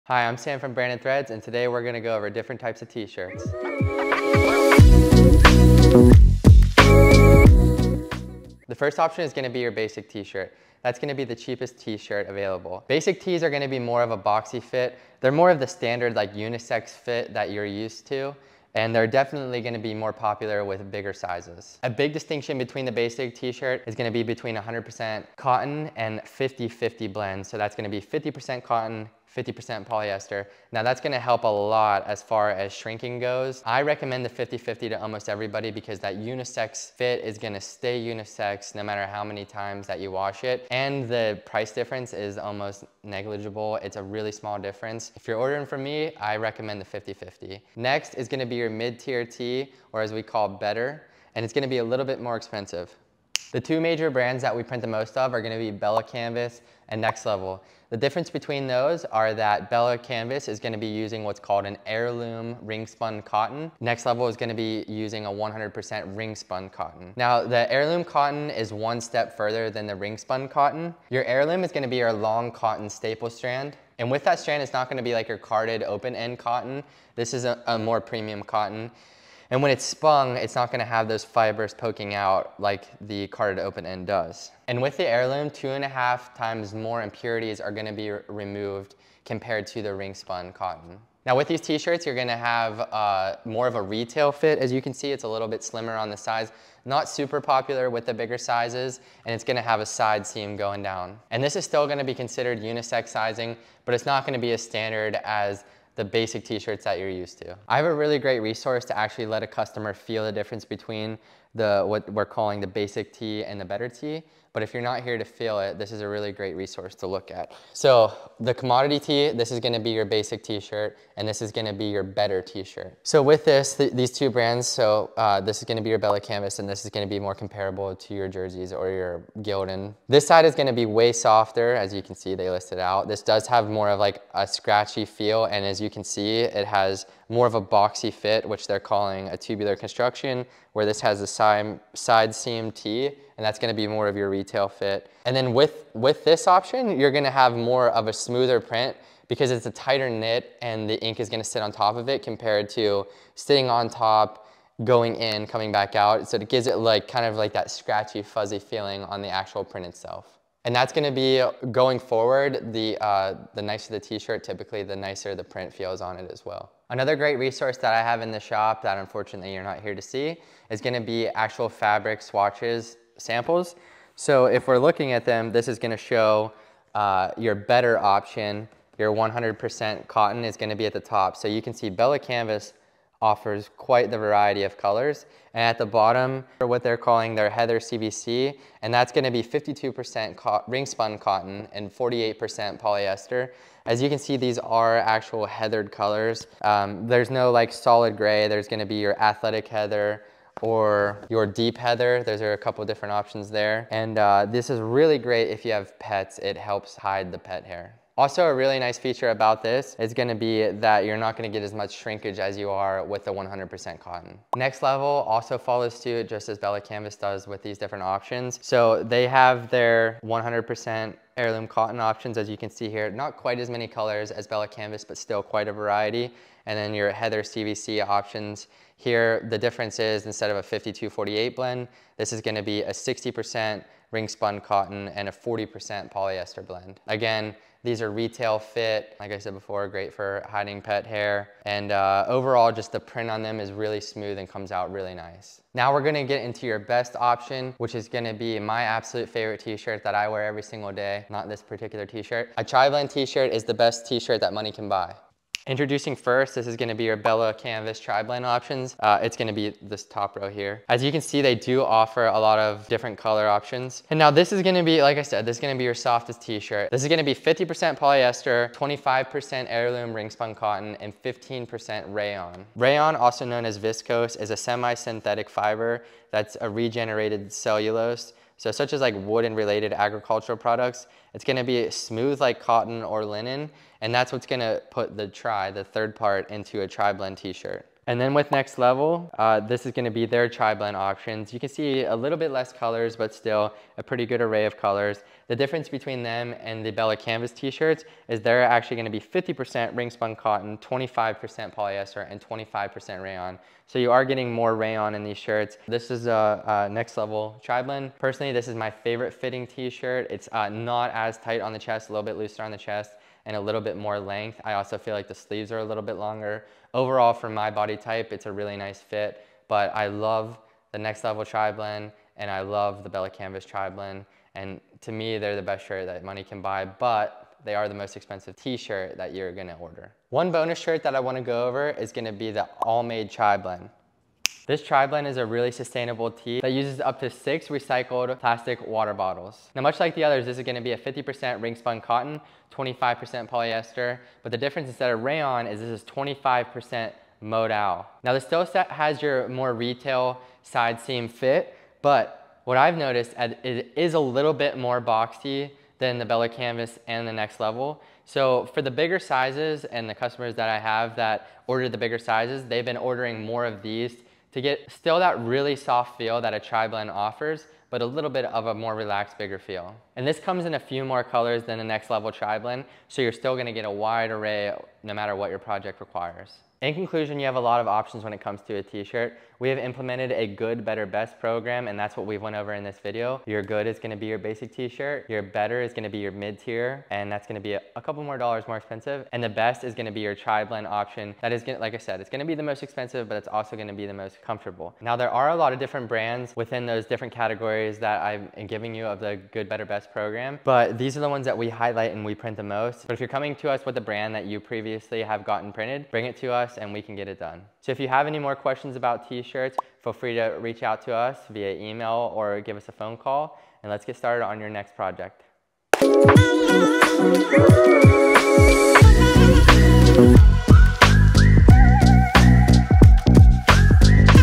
Which T-Shirt Is Right For You - A Guide
Today I’m going to teach you about different types of t-shirts to help find which one is best for you! We can’t wait to work together 🤩

Buy my brand that donates 50% of profits to mental health research